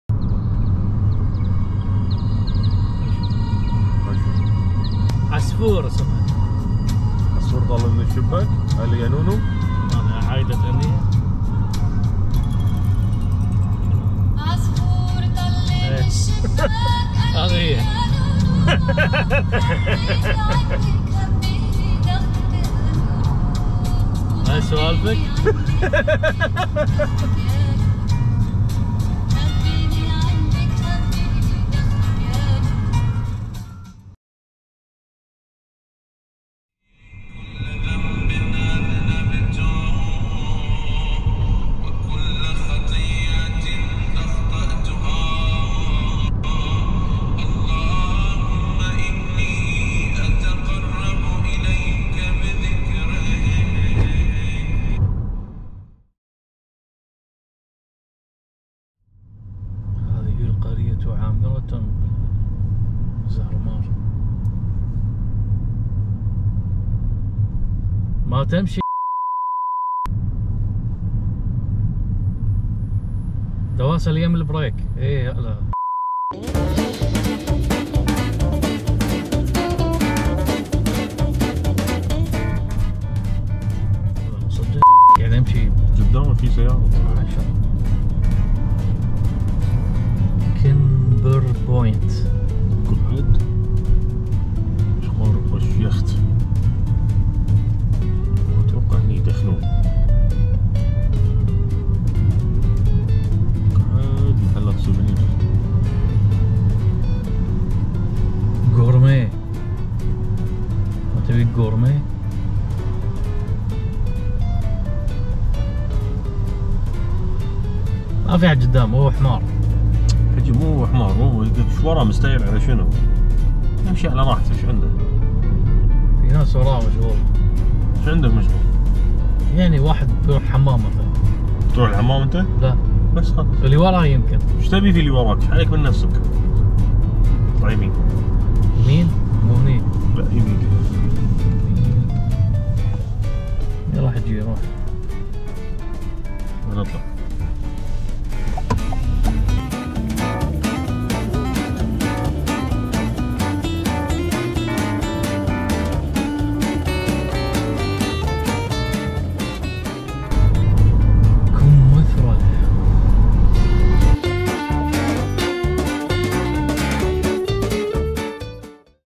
يوميات يحيى و بدر
يوميات يحيى و بدر ، سلسلة يوميات رحله طلعناها انا (بدر) و يحيى الي زارني من الكويت .. الرحله كانت من ولاية نورث كارولاينا الى 14 ولايه امريكيه  كانت مدتها 3 اسابيع .. متابعه ممتعه